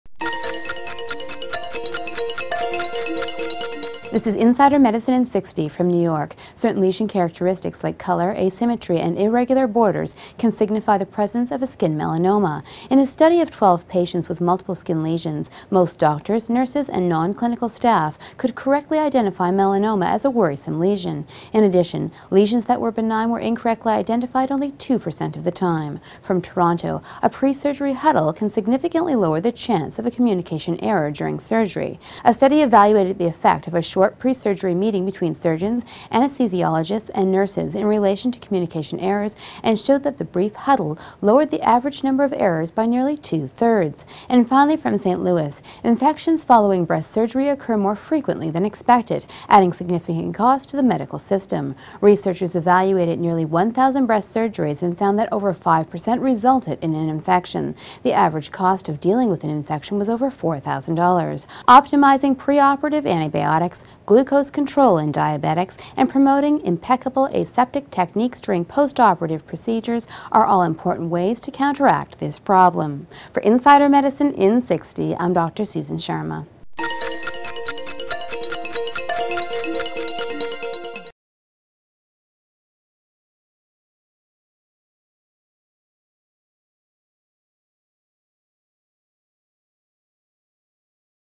Color, asymmetry, irregular borders can indicate skin melanoma - IN60
From New York - Certain lesion characteristics like color asymmetry and irregular borders can signify the presence of a skin melanoma. In a study of 12 patients with multpile skin lesions, most doctors, nurses and non-clinical staff could correctly identify melanoma as a worrisome lesion. In addition, lesions that were benign were incorrectly identified only 2% of the time.

From Toronto - A pre-surgery huddle can significantly lower the chance of a communication error during surgery. A study evaluated the effect of a short pre-surgery meeting between surgeons, anesthesiologists and nurses in relation to communcation errors and showed that the brief huddle lowered the average number of errors by nearly two thirds.

And finally, from St. Louis - Infections following breast surgery occur more frequently than expected, adding significant cost to the medical system. Researchers evaluated nearly 1,000 breast surgeries and found that over 5% resulted in an infection. The average cost of dealing with the infection was over $4,000. Optimizing pre-operative antibiotics, glucose control in diabetics, and promoting impeccable aseptic techniques during post-operative procedures are all important ways to counteract this problem.